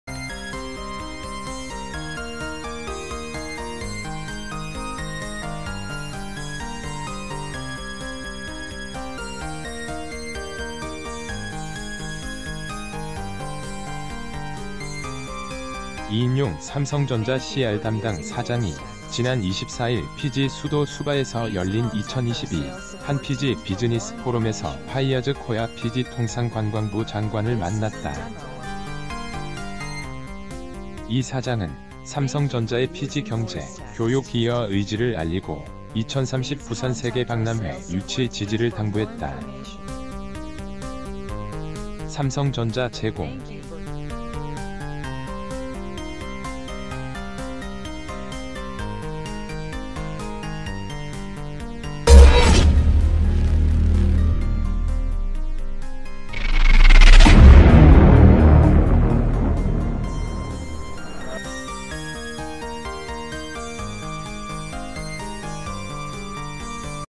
[포토] 이인용 삼성전자 사장, 피지 찾아…“부산세계박람회 유치 지지” 당부[포토] 이인용 삼성전자 사장, 피지 찾아…“부산세계박람회 유치 지지” 당부
[포토] 이인용 삼성전자 사장, 피지 찾아…“부산세계박람회 유치 지지” 당부 이인용 삼성전자 CR담당 사장(오른쪽)이 지난 24일 피지 수도 수바에서 열린 ‘2022 한·피지 비즈니스포럼’에서 파이야즈 코야 피지 통상관광부 장관을 만났다. 이 사장은 삼성전자의 피지 경제·교육 기여 의지를 알리고 ‘2030 부산세계박람회’ 유치 지지를 당부했다. 삼성